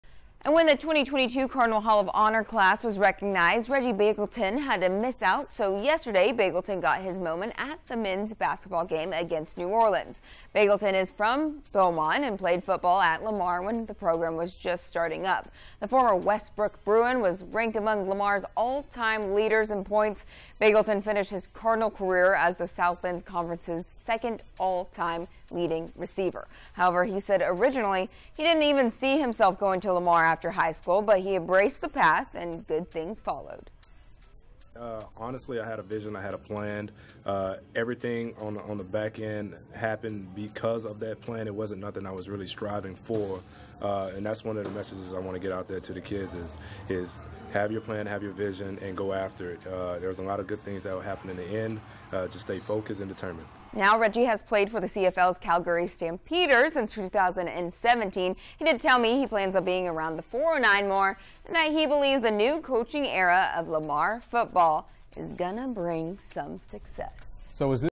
Lamar Athletics recognizes Reggie Begelton at basketball game
Begelton was a 2022 Hall of Honor inductee.